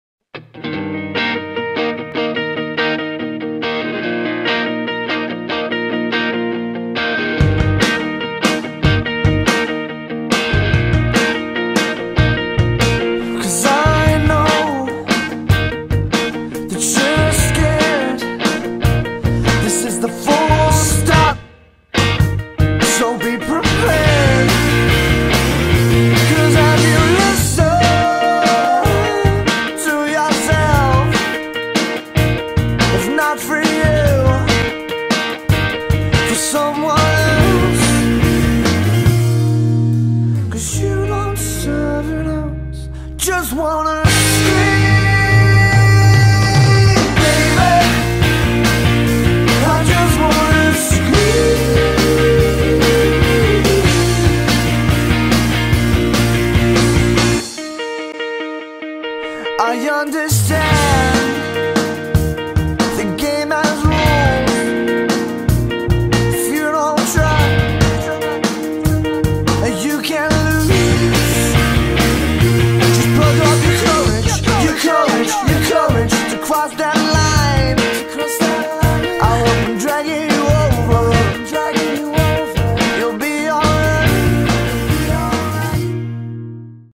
Millies Cookies at T4 on the Beach!
Our client Millies Cookies had lots of fun with the stars at T4 on the Beach this summer. Including David Hasslehoff, McFly and the T4 presenters.